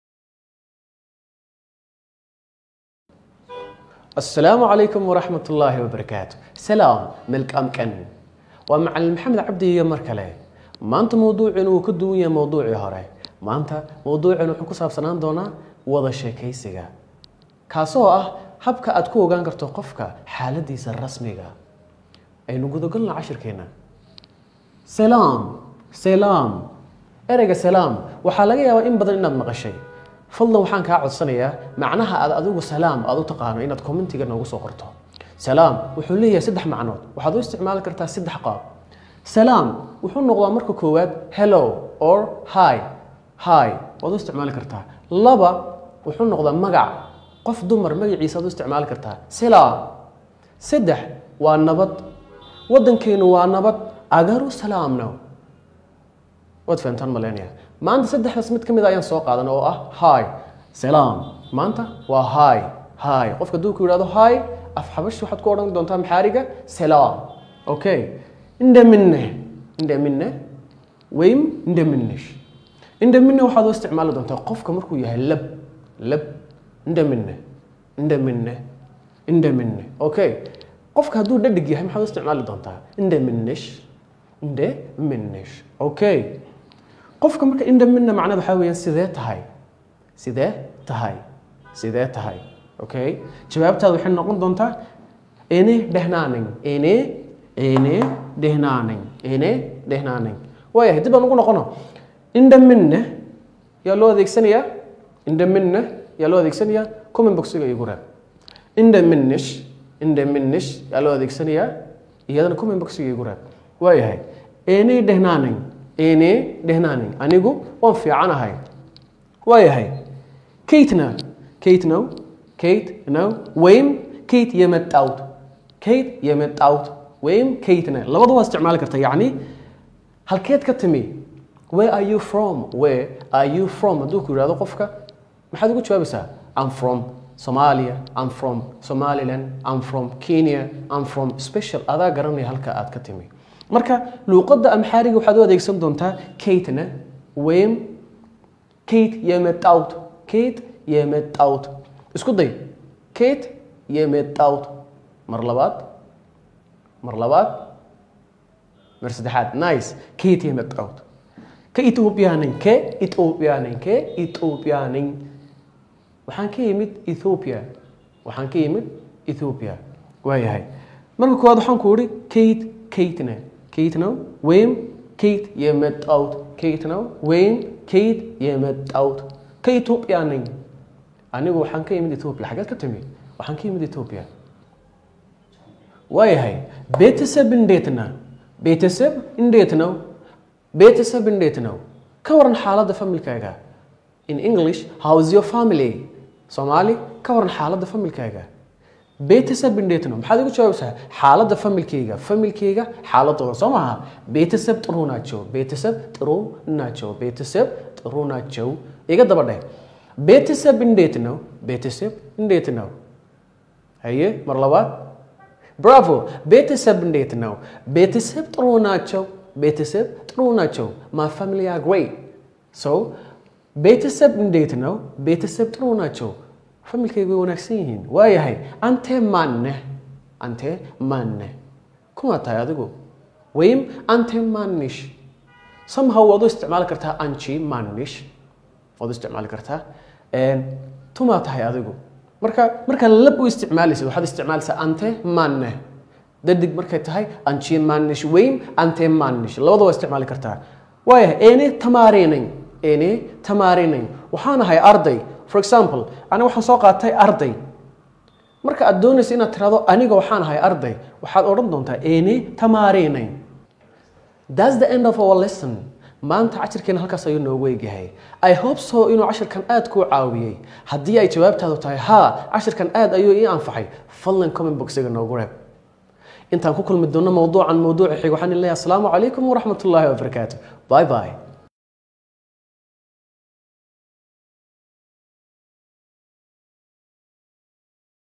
Salaanta Af-amxaariga - Macalin Maxamed Abdi.
Salaanta Af-amxaariga - Macalin Maxamed Abdi. Haddii aad jeceshahay in aad online ku barato Af-amxaariga, fadlan comments-ka inoogu soo qor ama la xidhiidh Whatsapp-ka Hodan